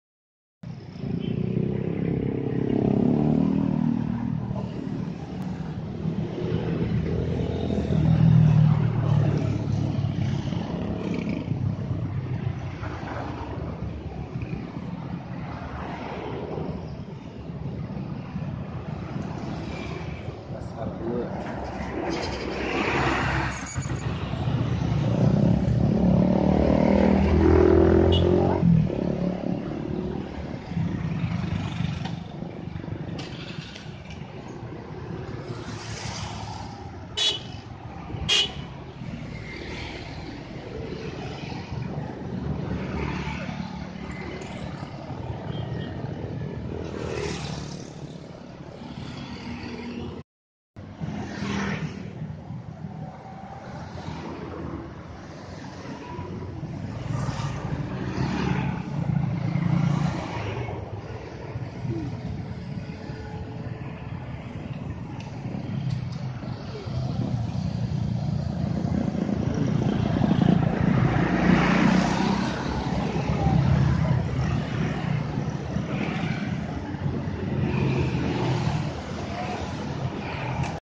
Keren! Bis "Hiba Utama" New Legacy SR2 HD Prime Hino Laksana
Video Bis "Hiba Utama" New Legacy SR2 HD Prime Hino Laksana di jalan lurus dan tikungan bumiayu.